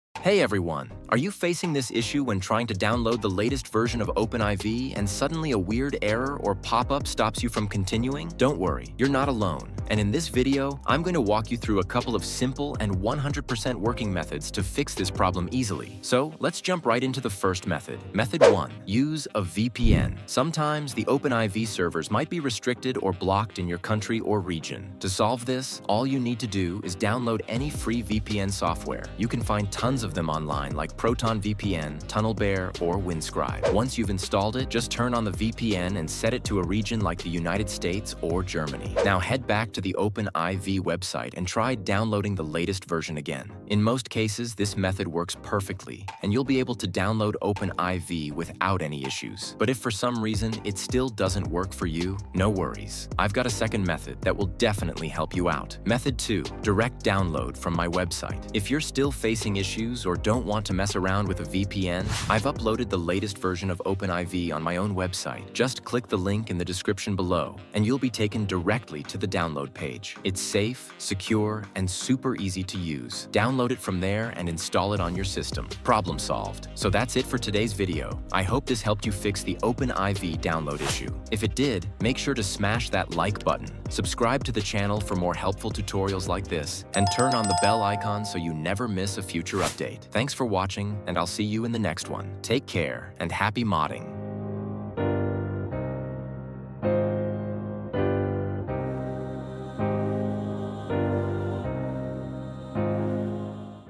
How To Fix Open IV Not Downloading (2025) | Invalid SSL certificate Error Fix | GTA 5 Mods
Title : How To Fix Open IV Not Downloading (2025) | Invalid SSL certificate Error Fix | GTA 5 Mods

Videos Chapters :
00:00 intro
00:18 Method 1
00:55 Method 2

PC Spec's
Processor : Ryzen 5 5600
Motherboard : Msi B450m A-Pro Max
Gpu : XFX Rx 6600xt
Ram : Delta T-Force 16gb
PSU : Corsair TX750